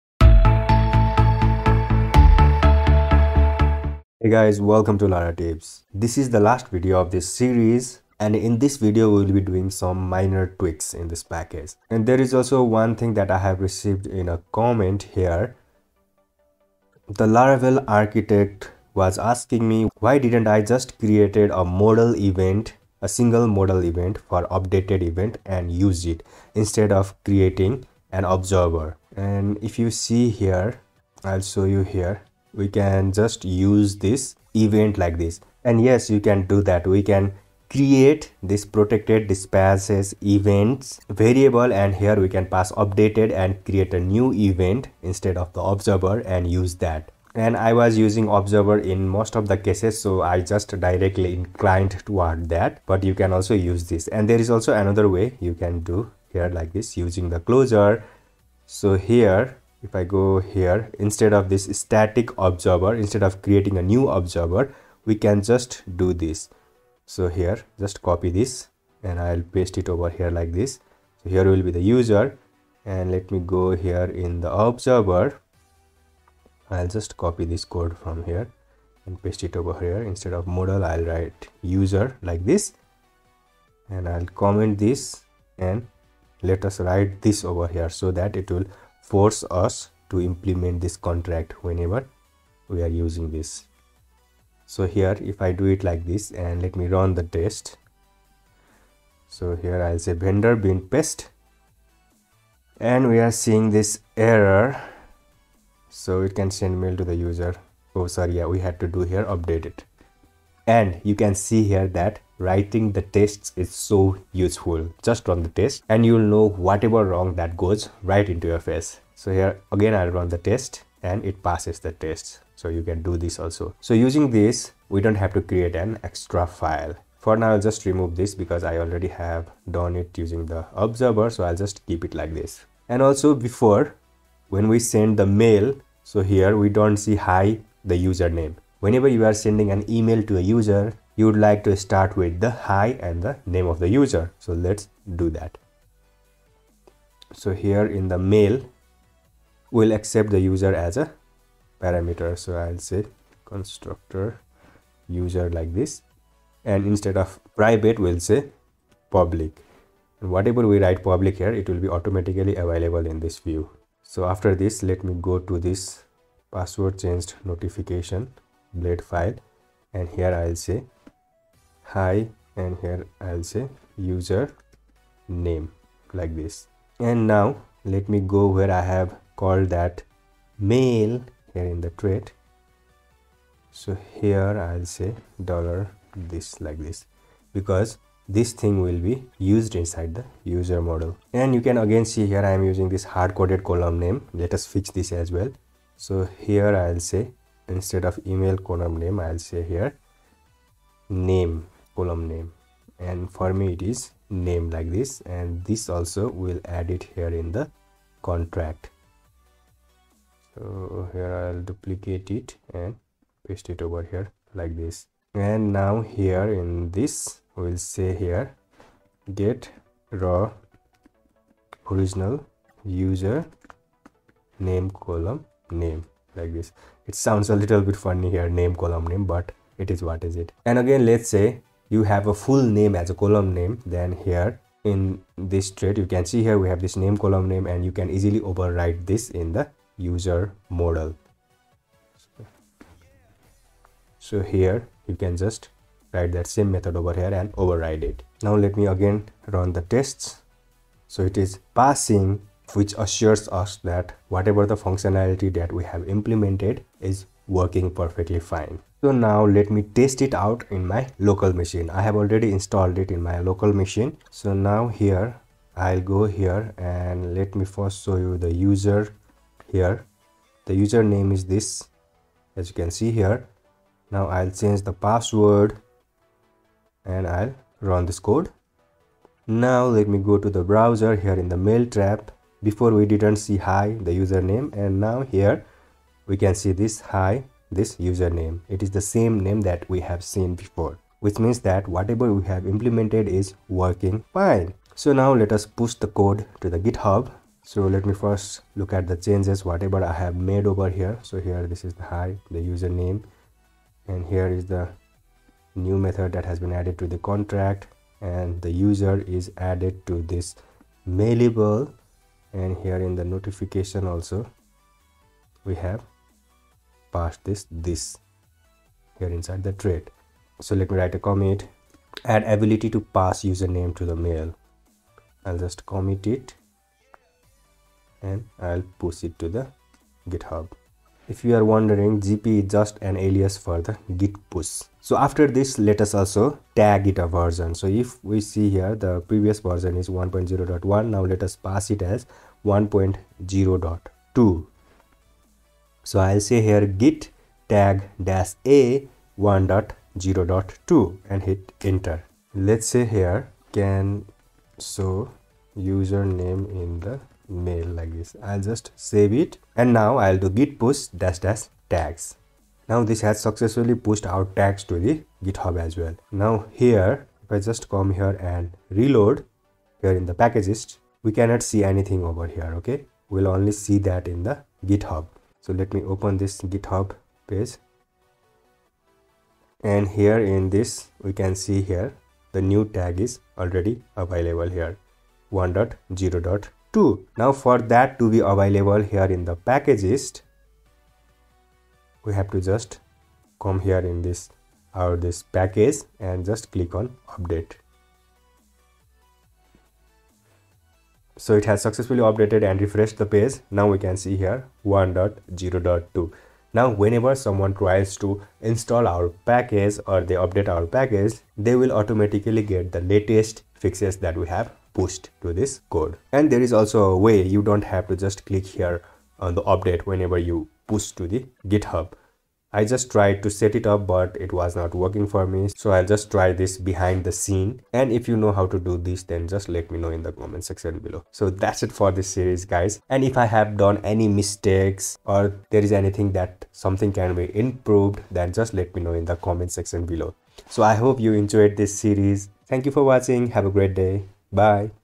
Laravel Package Development - Final - Part 5
This is part 5(final) of the Laravel package that we will be developing. It will automatically send mail to the user when the password changes. I hope you loved this series as well.

Tech stack

Laravel
PHP

Photo source